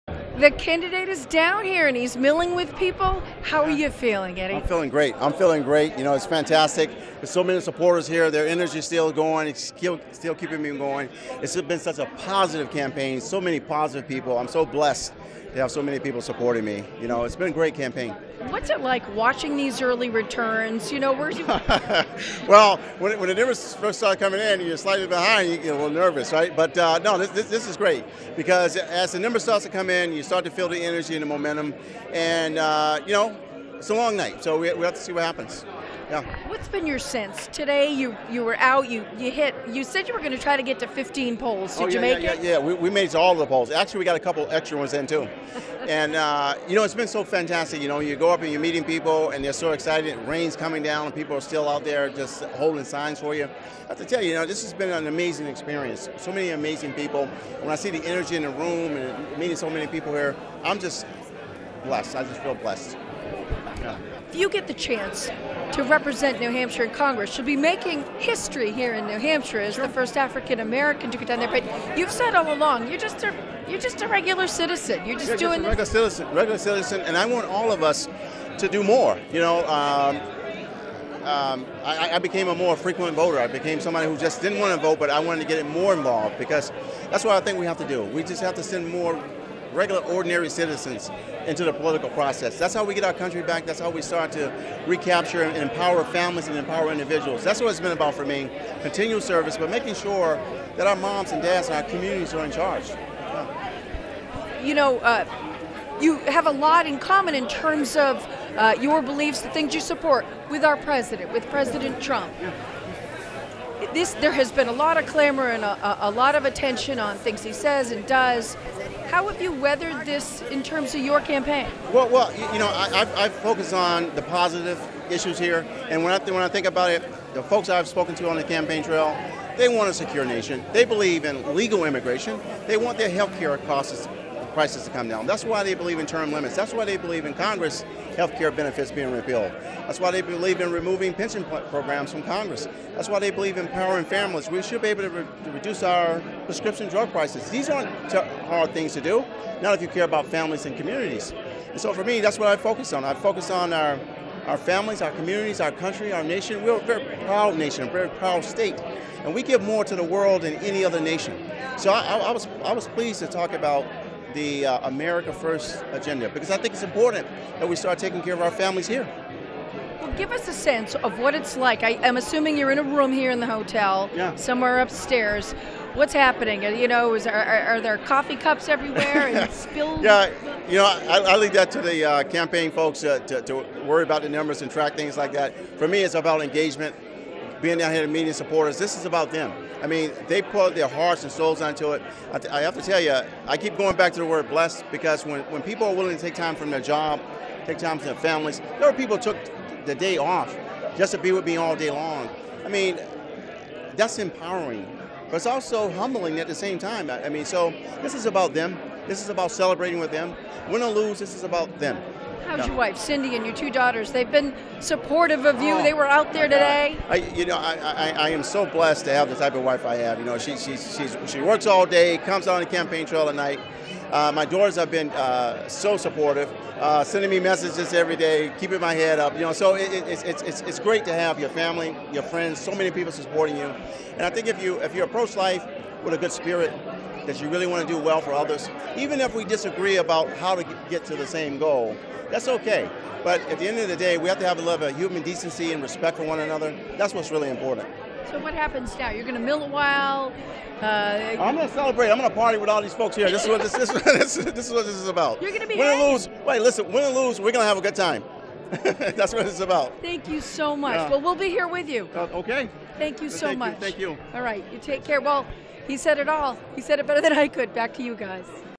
Eddie Edwards feeling 'fantastic' as results pour in
1st District Republican Eddie Edwards said he's feeling "great" as results continue to pour in.